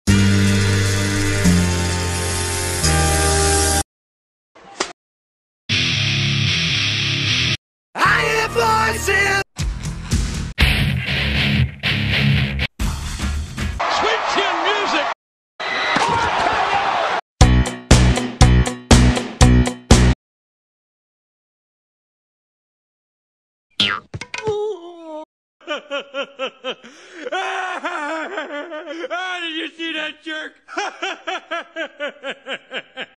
PWE: John Cena Getting Owned
I was bored again and made an animation where everyone kills cena. His finisher is weak as hell and he just plain sucks. (Watch in High Quality)